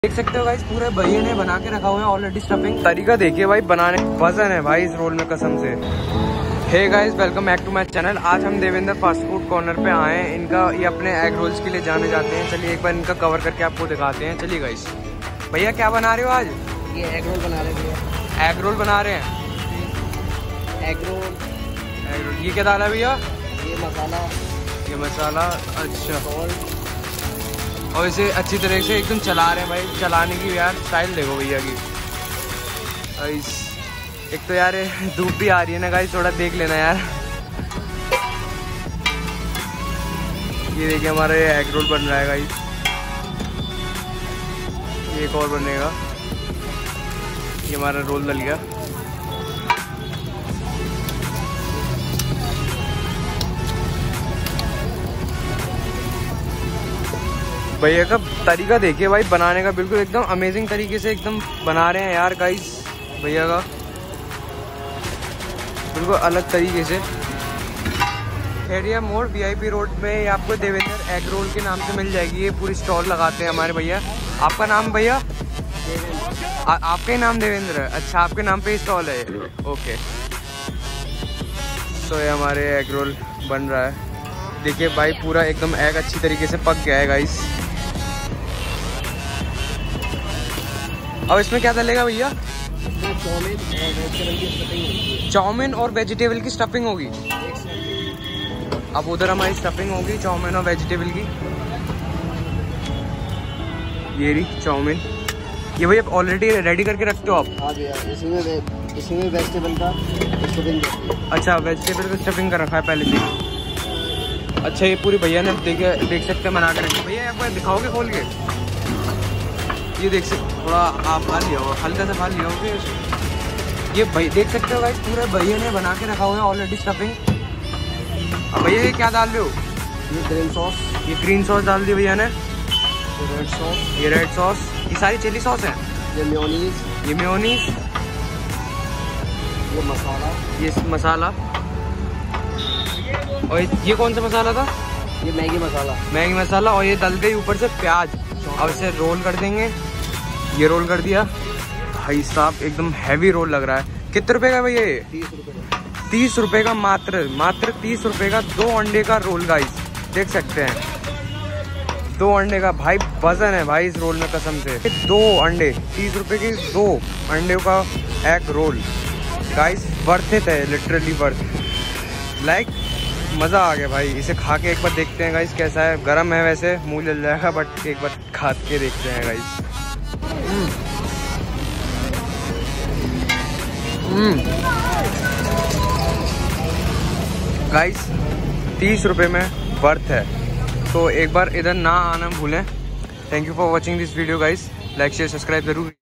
Egg Roll With Chowmien | Agra’s Popular Anda Roll | Indian Street Food
A hardworking man selling egg rolls in Agra
Location - Devendra egg roll,kheria mod,vip road,Agra